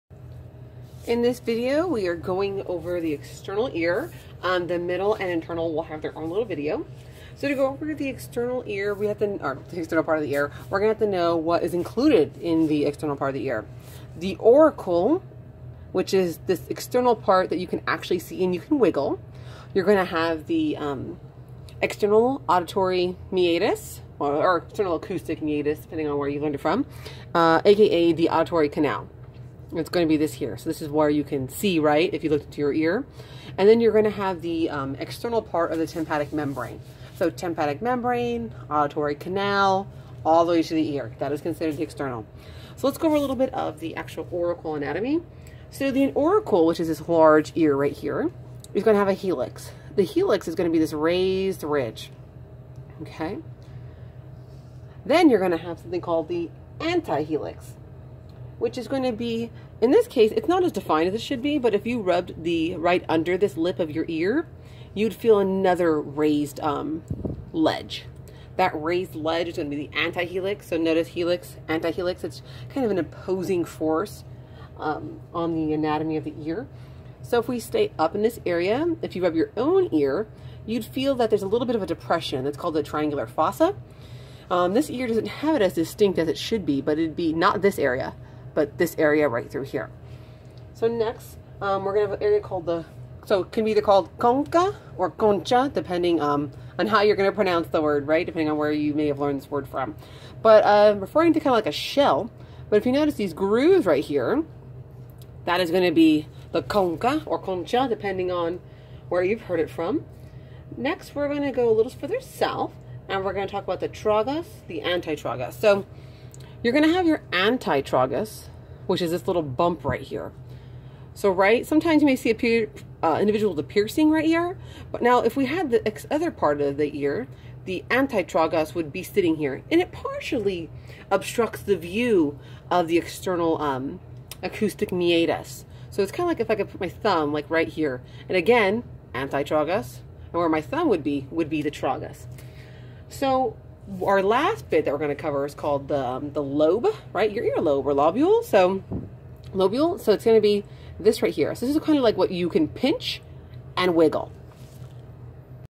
External Ear
Brief overview of the external ear on the model- helix, triangular fossa, antihelix, concha, external acoustic meatus, antitragus, lobule (earlobe), auditory canal, and auricle.